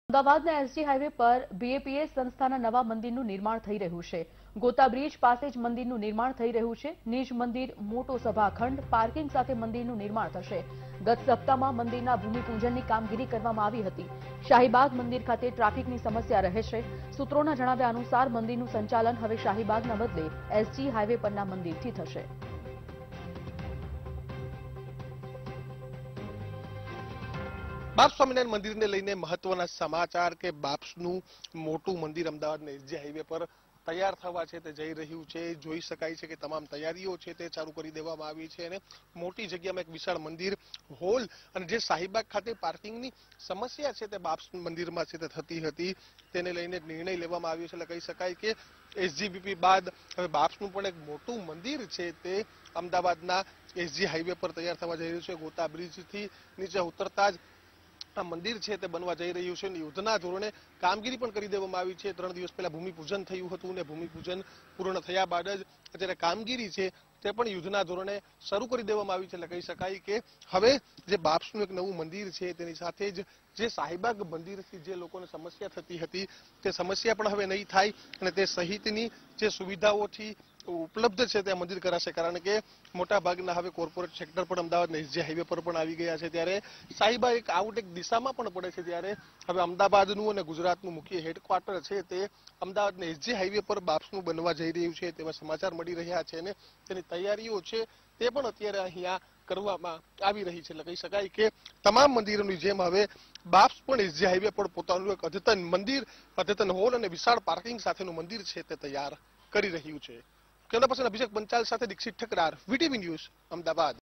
Ahmedabad ના SG હાઇવે પર BAPS સંસ્થાના નવા મંદિરનું નિર્માણ, ગત સપ્તાહમાં ભૂમિપૂજનની કરાઈ કામગીરી
Ahmedabad ના SG હાઇવે પર BAPS સંસ્થાના નવા મંદિરનું નિર્માણ, ગત સપ્તાહમાં ભૂમિપૂજનની કરાઈ કામગીરી | VTV GUJARATI